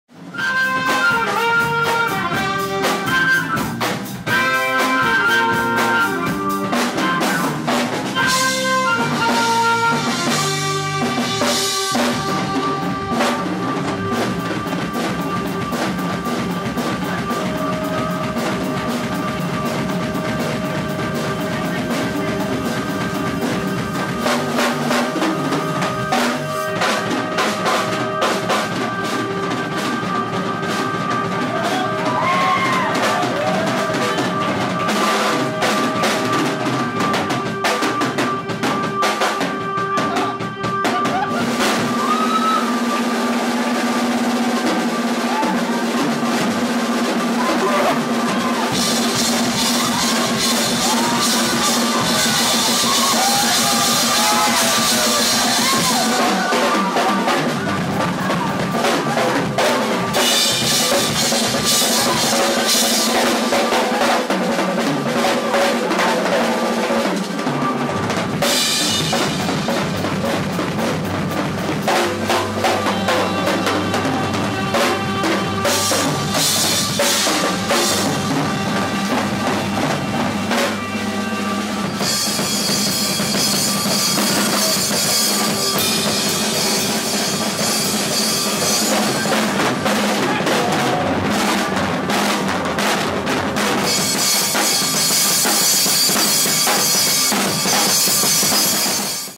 AMAZING HEAD TO HEAD DRUM-OFF @ EDNA MANLEY SCHOOL OF MUSIC
Willie Stewart and a student of Edna Manley's School of Visual & Performing Arts in Jamaica go head to head in showing who's got skills! Amazing footage of a true legend in action.